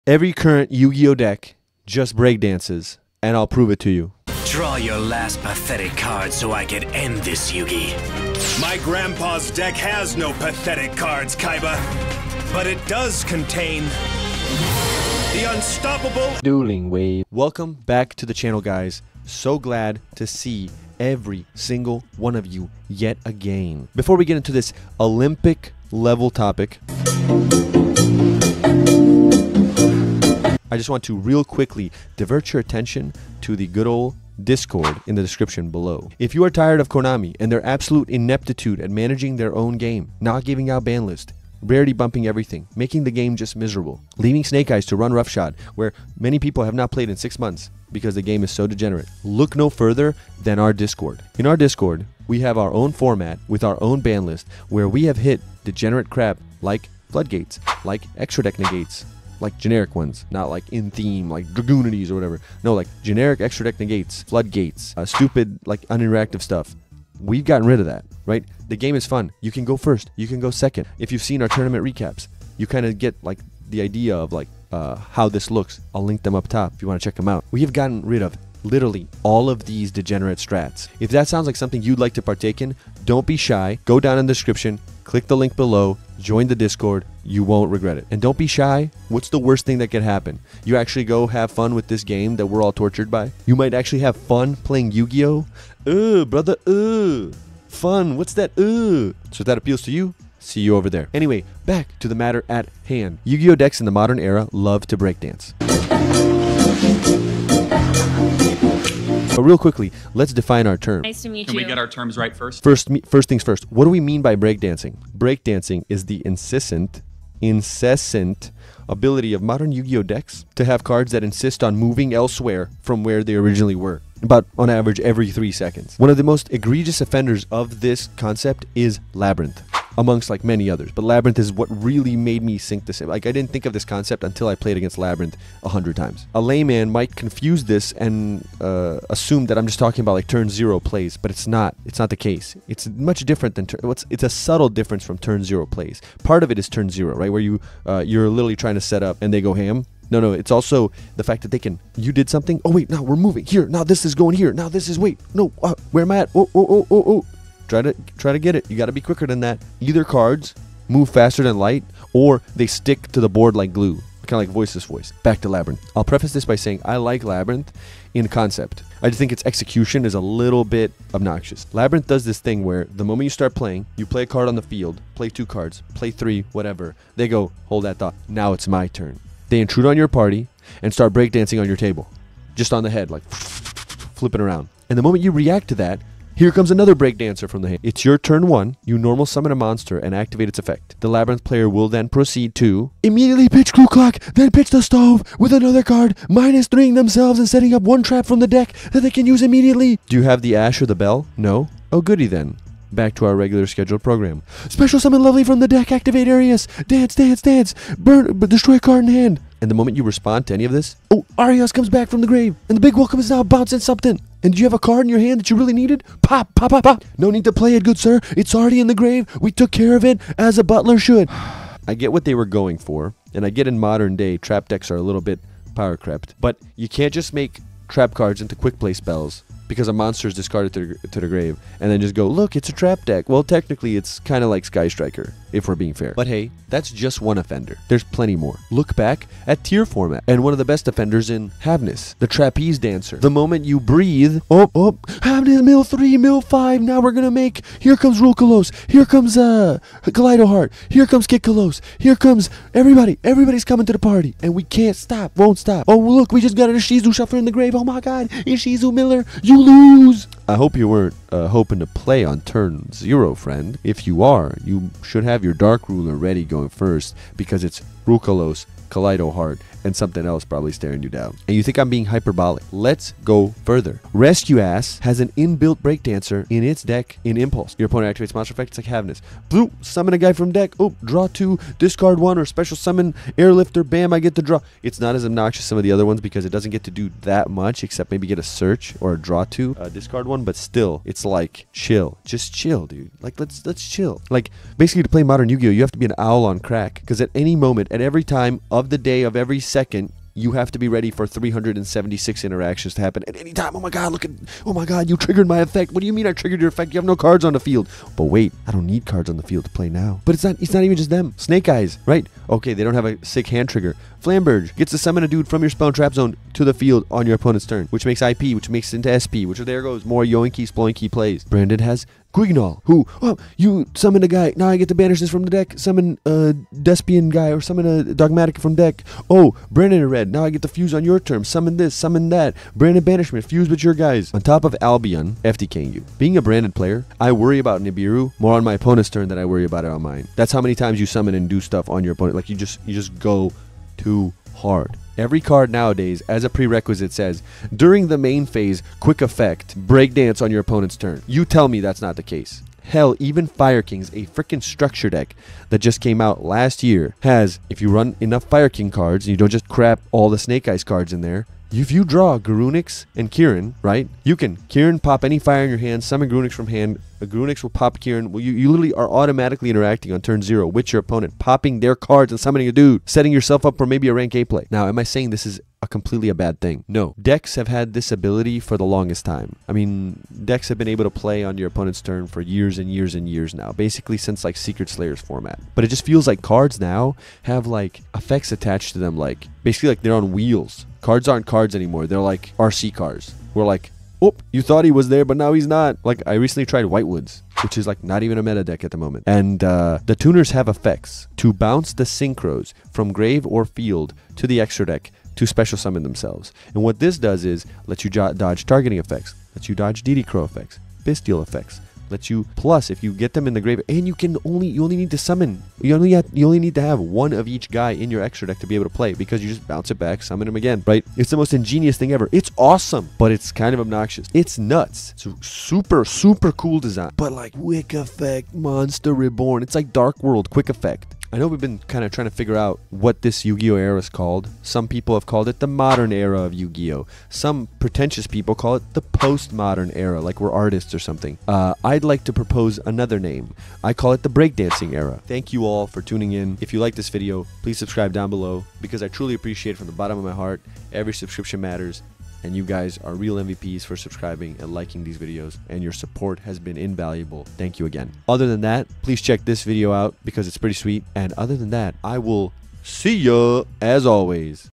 Every modern Yugioh deck insists on breakdancing.
Today i want to talk about how every yugioh deck in the game insits on breakdancing and im tired of it, these cards need to sit still while i go off.